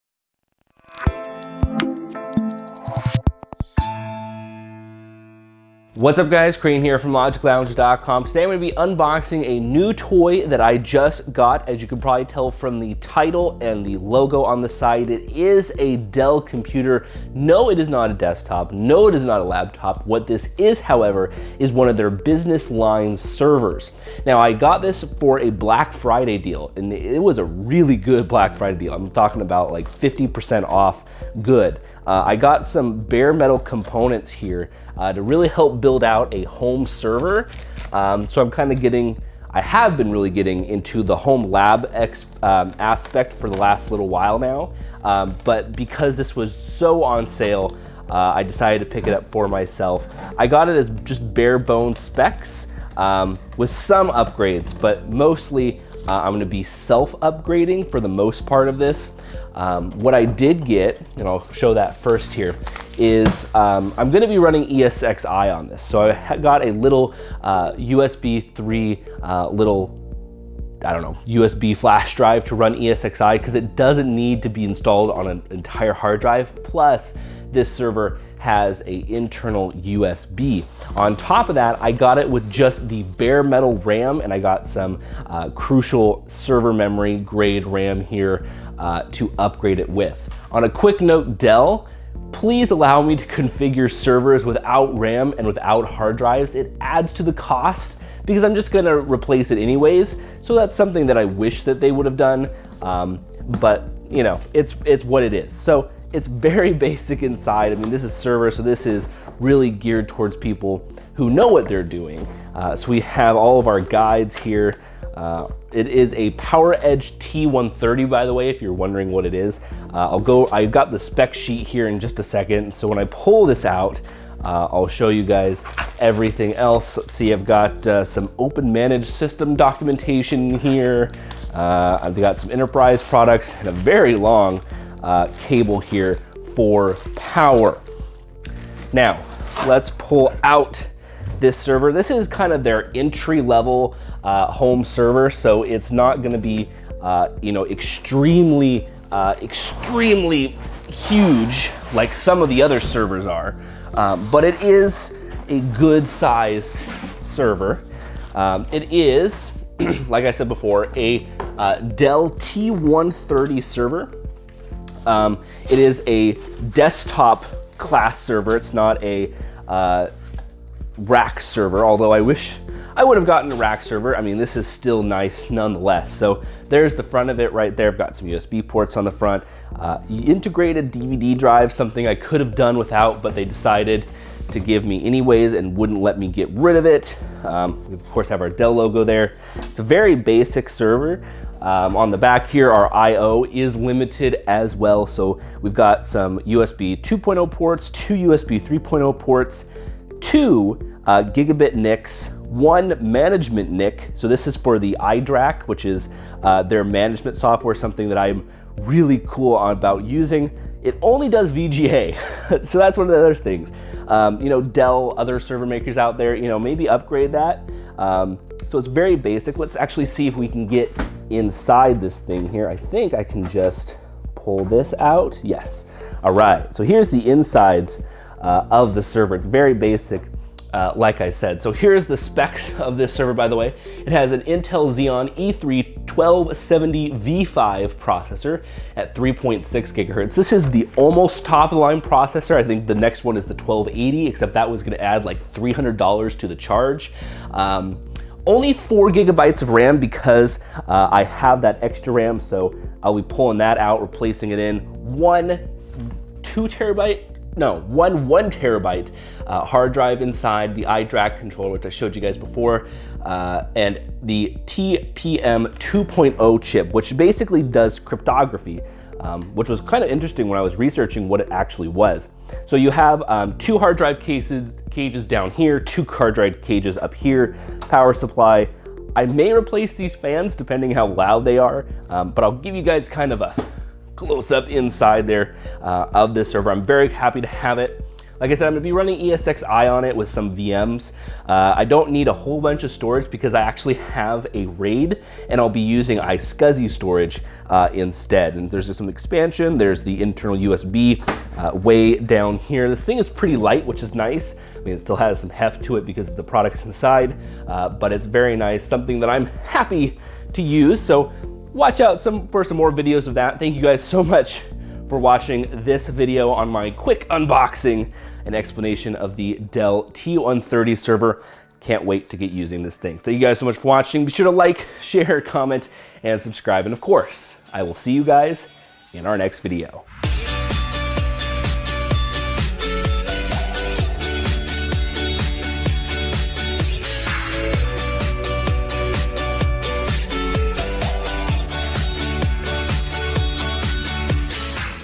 Unboxing - Dell PowerEdge T130 Tower Server
After toying around with the idea of getting a home lab server I got a great Black Friday deal on a Dell T130 which is the perfect size for a home server.

SOCIAL